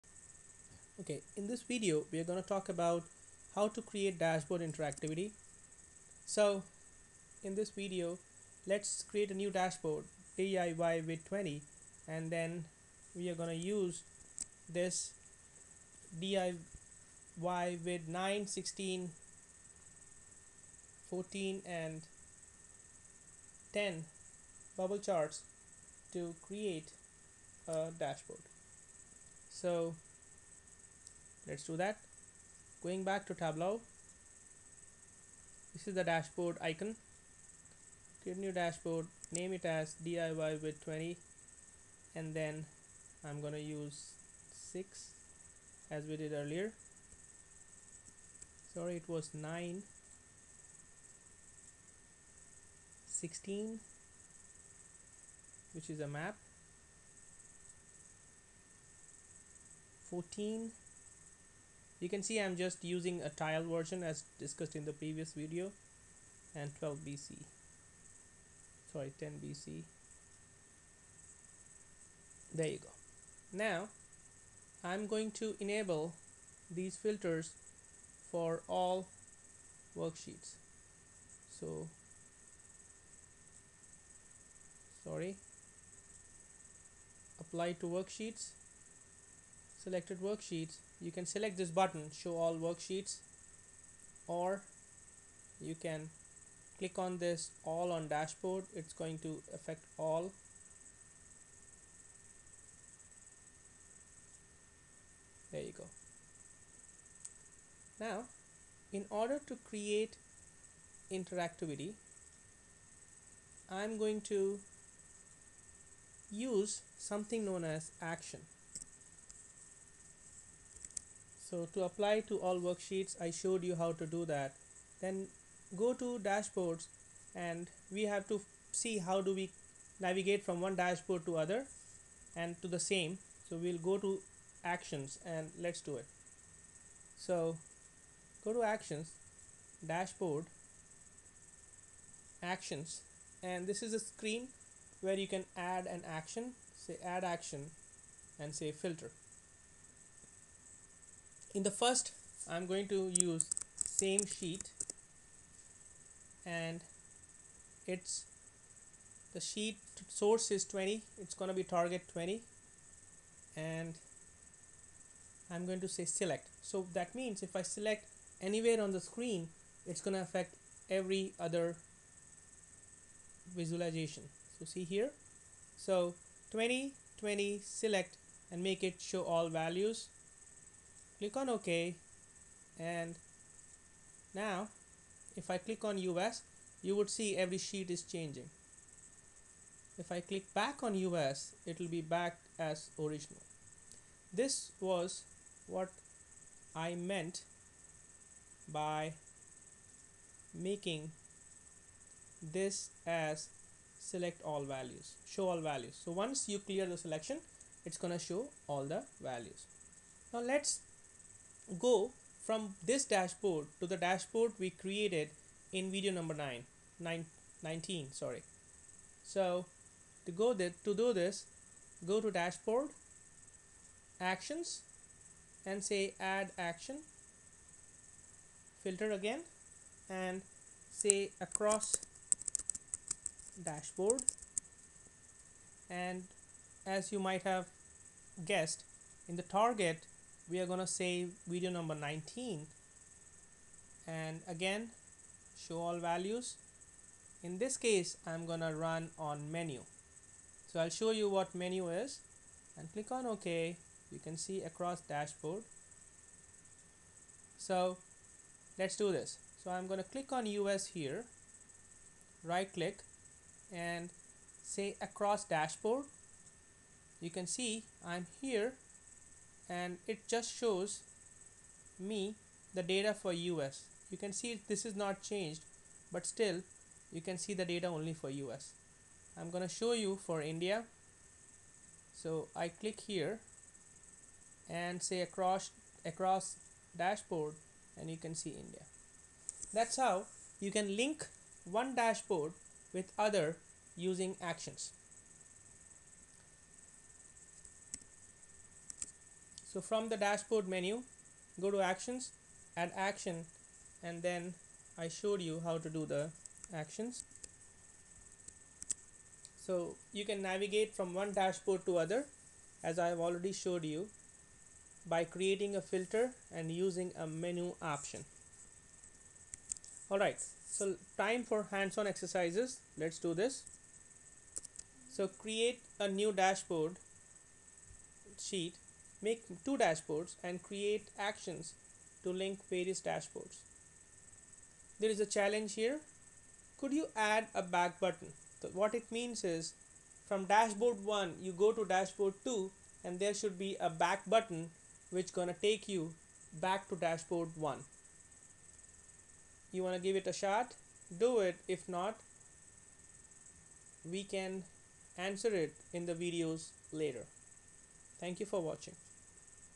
Tableau - Do it Yourself Tutorial - Dashboard Interactivity - DIY -20-of-50
Tableau - Do it Yourself(DIY) Tutorial - Dashboard Interactivity - DIY -20-of-50
by

In this video, we will see how to introduce interactivity in Dashboards created in Tableau.
1- Open the saved TWB file - Tableau BDCS DIY.
2- Use the data connection from the Video-1 of the Tableau DIY series
3- Create a new Dashboard - DIY - Vid - 20 using DIY Vid - 9/ 16/ 14/ 10-BCs
4- To apply a particular to all the worksheets, click on the filter and change the type of the filter.
5- Let's navigate from the dashboard to other sheets. To do this, enable Actions.
6- From the dashboard menu, goto Actions, and Add an action.
7- Add an Action for the same dashboard.
8- You can also, navigate from one Dashboard to other, if you have multiple dashboards.
9- Create a new action, and create a filter to navigate from this Dashboard, to the dashboard created DIY - Vid - 19.
10 Create a back button.
11- Let's do the following Hands-on exercises.

Hands on - DIY #33
1- Add a new dashboard sheet.
2- Create 2 dashboards, and create actions to link various dashboards.
3- Add an Action for the same dashboard.
4- Add an Action for across dashboards.
5- Add a back button.